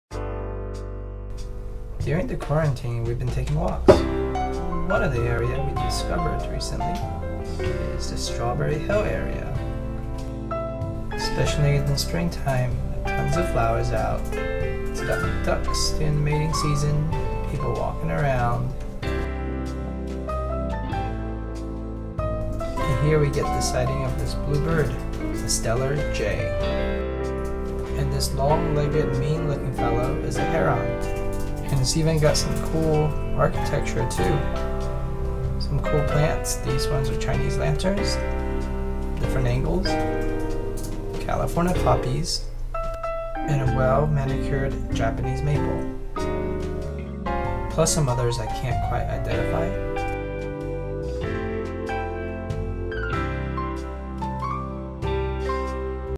Golden Gate Park - Strawberry Hill
Quick walkaround of Strawberry Hill during covid-19.
May 11 6pm 2020